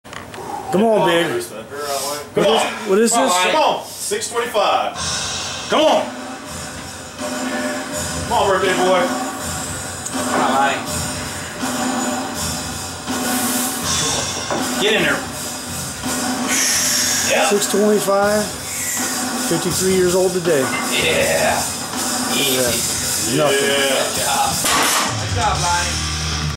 Lonnie 625 deadlift
Getting some pulls in on his birthday!  possible opener for upcoming UPA meet.